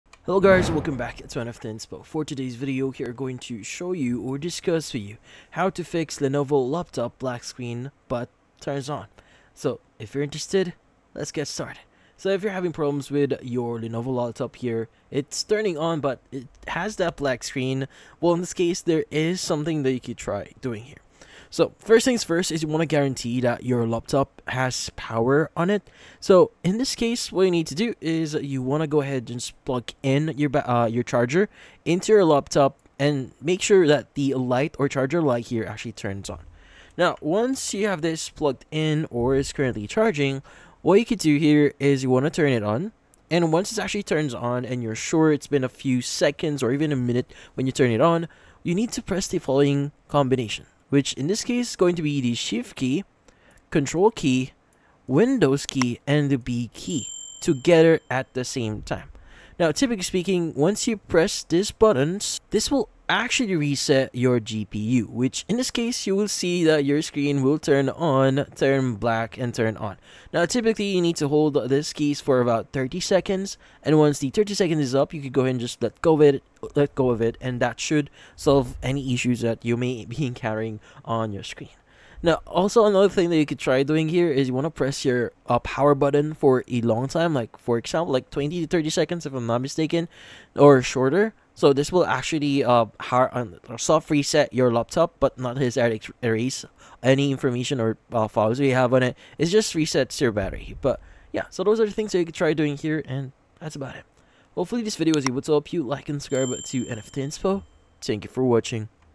How To Fix Lenovo Laptop Black Screen But Turns On
If you want to be able to fix lenovo laptop black screen but turns on, then this video will be perfect for you!